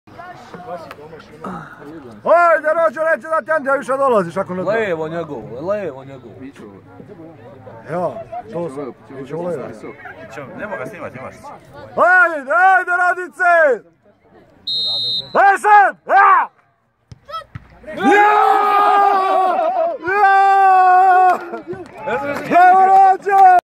Comi vs Picin
Pogledajte kako ne treba sutati penal.